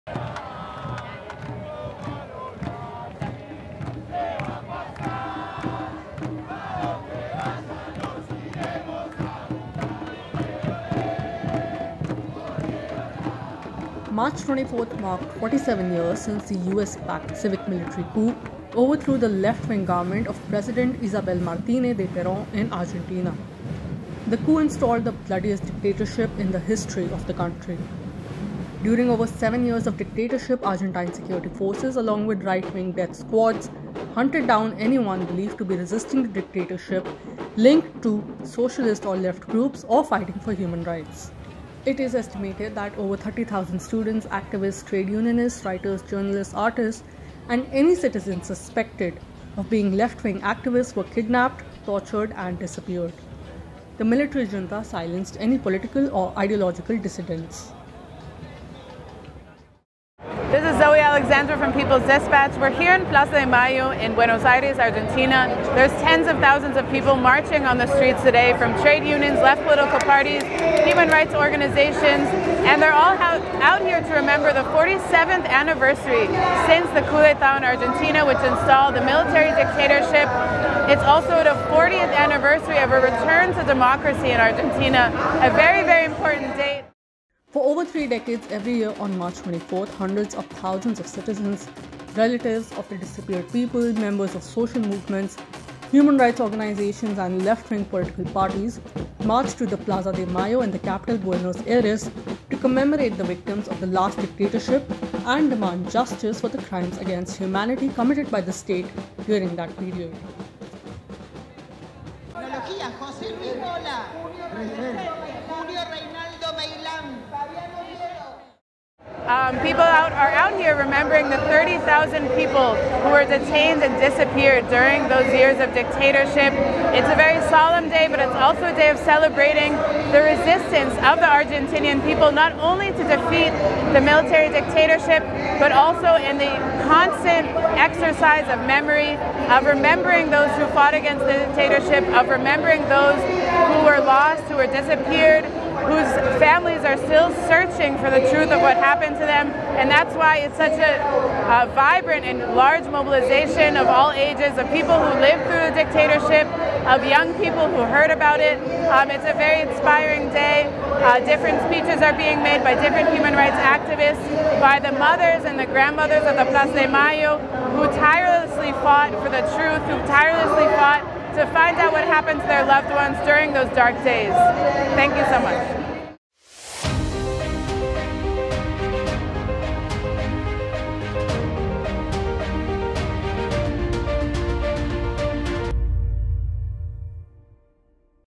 Argentinians on the streets at 47 years since the US-backed coup
March 24 marked 47 years since the US-backed civic-military coup overthrew the left-wing government of President Isabel Martínez de Perón in Argentina. The coup installed the bloodiest dictatorship in the history of the country. For over three decades, every year, on March 24, hundreds of thousands of citizens, relatives of the disappeared people, members of social movements, human rights organizations and left-wing political parties march to the Plaza de Mayo in the capital Buenos Aires to commemorate the victims of the last dictatorship and demand justice for the crimes against humanity committed by the State during that period.